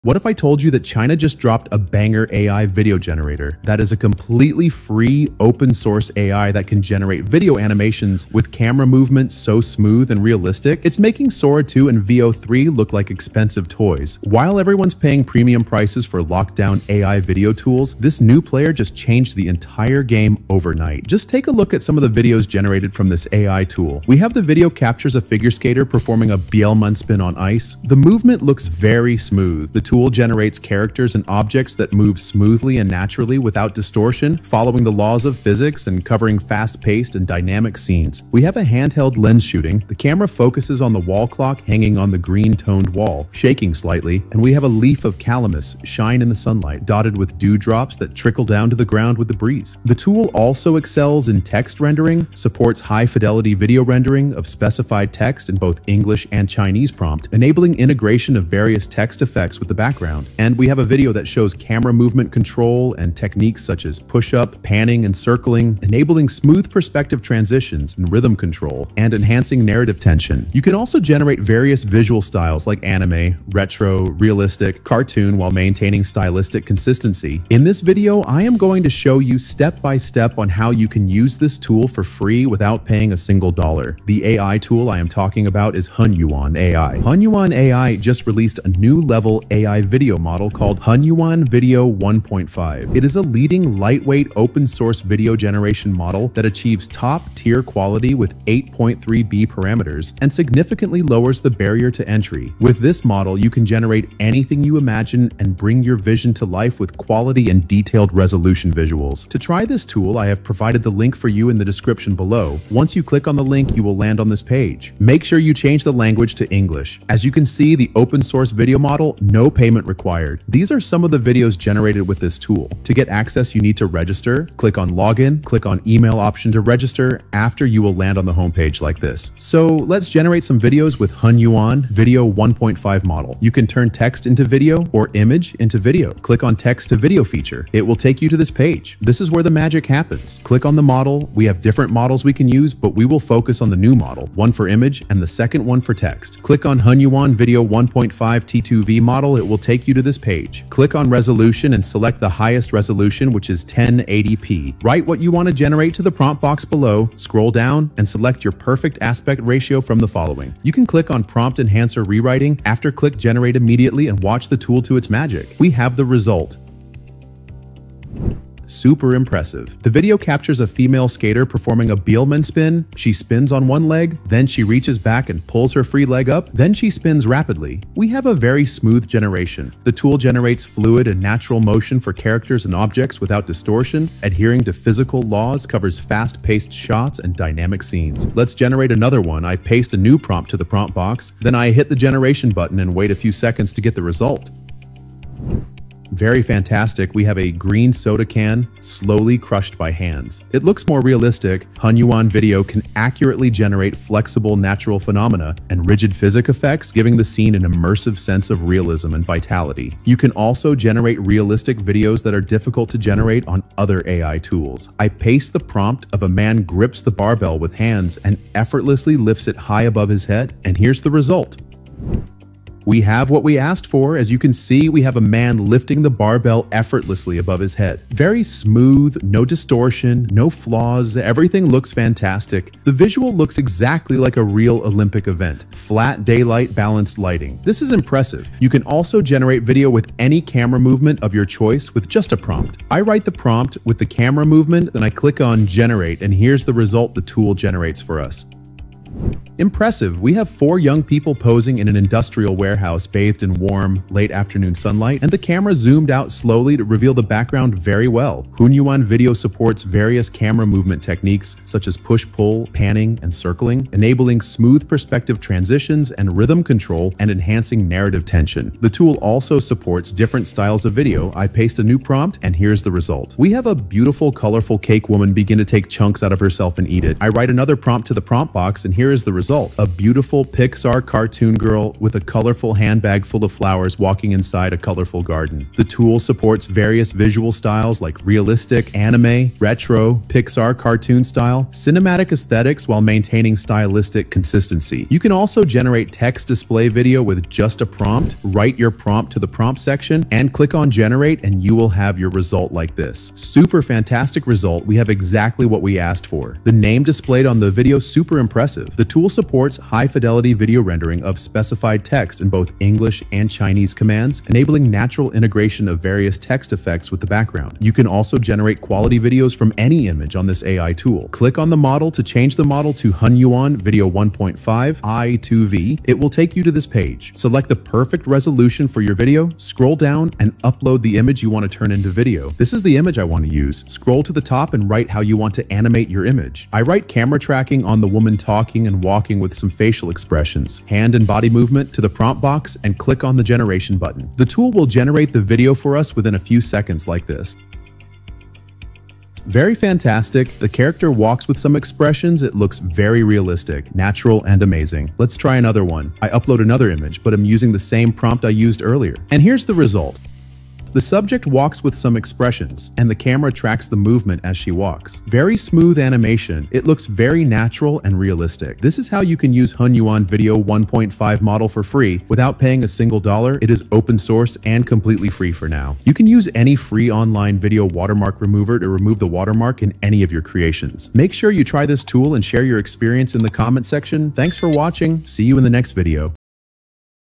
NEW OPEN SOURCE AI VIDEO RANKS NO 1 Beats Sora 2: Create UNLIMITED Videos for FREE (100% UNLIMITED)
Text to video | image to video | best new ai video generator | sora 2 | veo 3.1
Discover the power of the free AI video generator with Hunyuan Video 1.5 in this easy-to-follow tutorial! Learn how to create stunning, professional-quality videos effortlessly using the latest Hunyuan Video 1.5 AI technology—completely free. Perfect for content creators, marketers, and anyone looking to boost their video production without breaking the bank.

In this video, we guide you step-by-step through the setup, features, and tips to maximize your results with this incredible AI video generator.